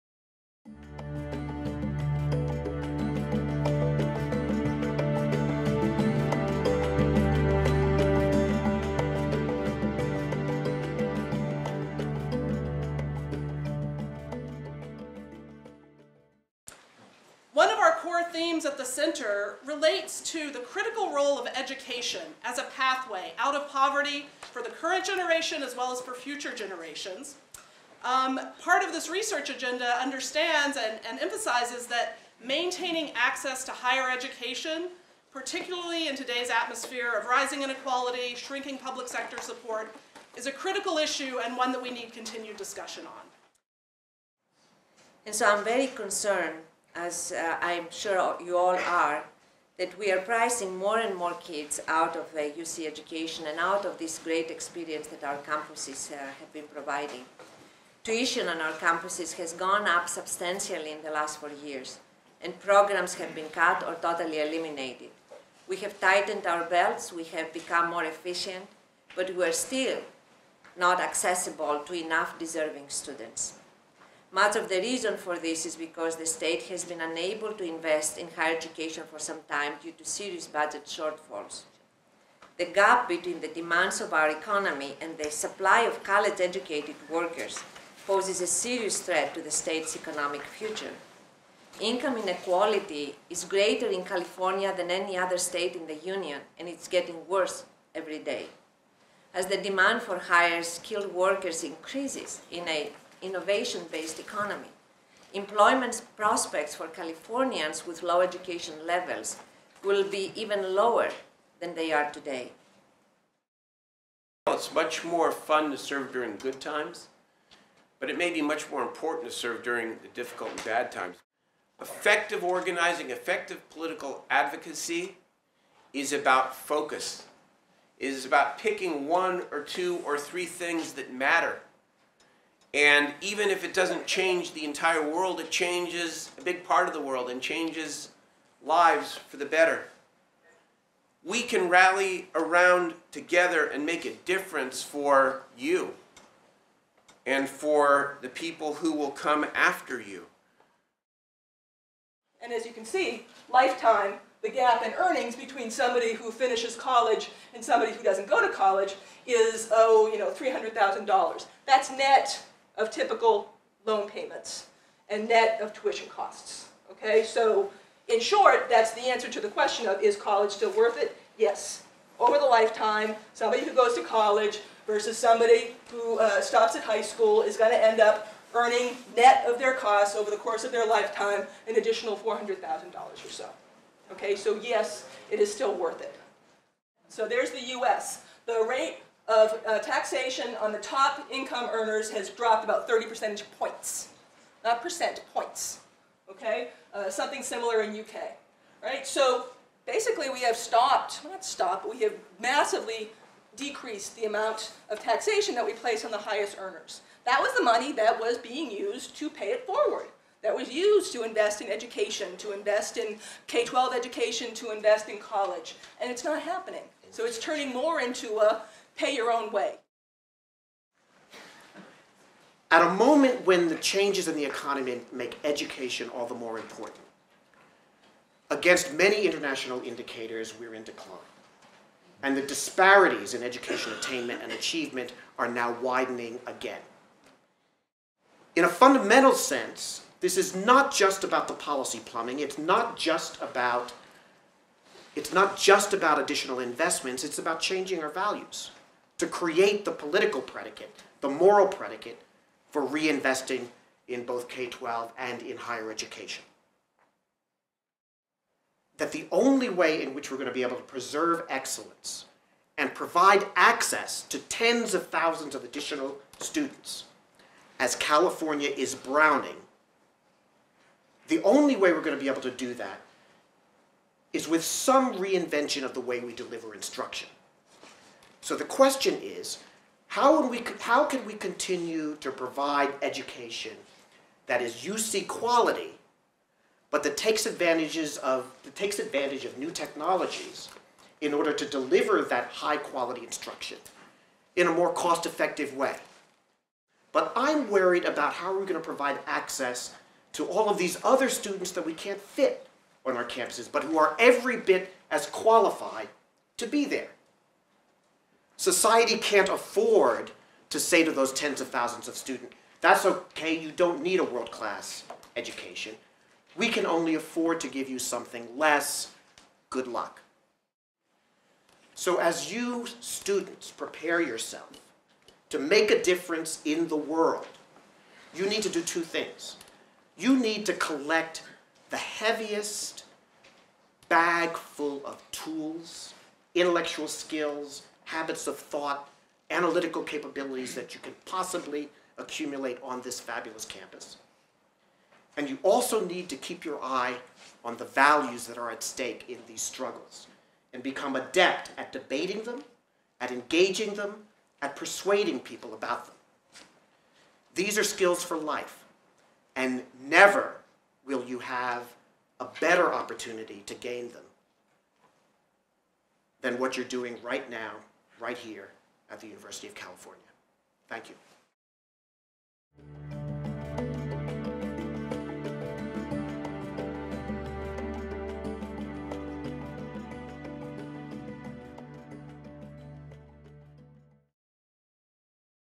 Inequality, Educational Access and the Future of the University
Panel of scholars and politicians discussing educational inequality and access at the UC Davis Center for Poverty Research event on January 20, 2012.